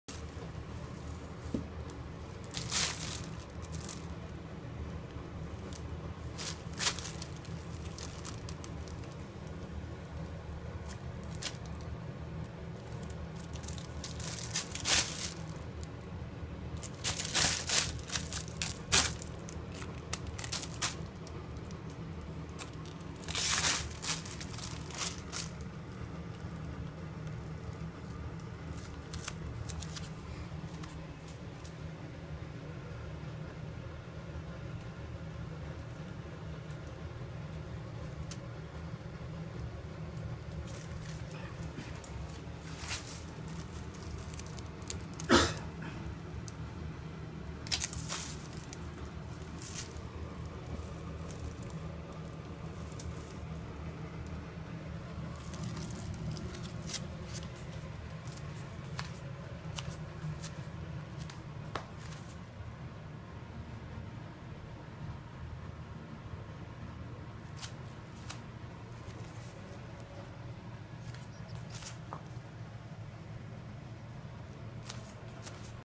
Mei brincando no saco 30-11-2014
Mei brincando no saco e me viu gravando e veio espia.. Haha!!! Fofinha